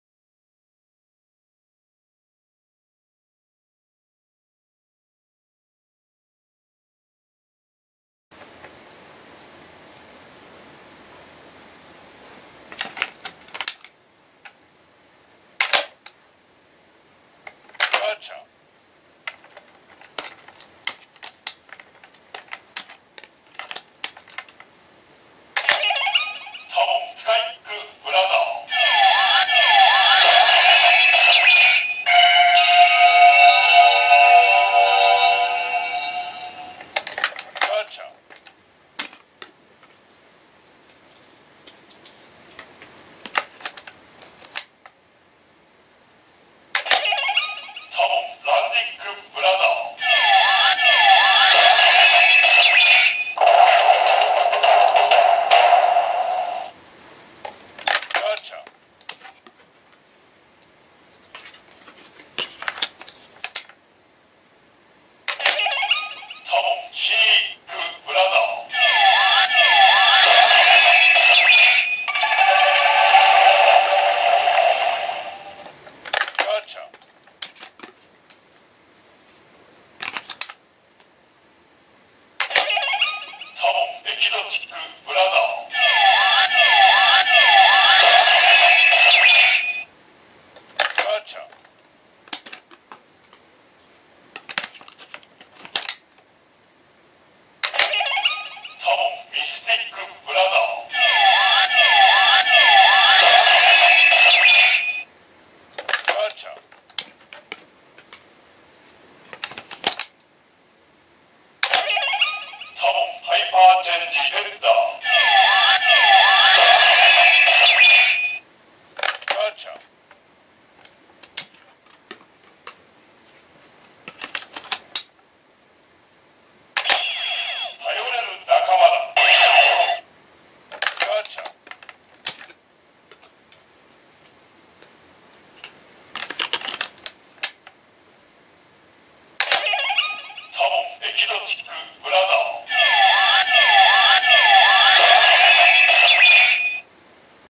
天装戦隊ゴセイジャー テンソウダー ブラザー系カード編 Goseiger Tensouder
天装戦隊ゴセイジャーより、ゴセイパワー開放器テンソウダーの ブラザー系カード編です。

■テレビマガジン2010年9月号付録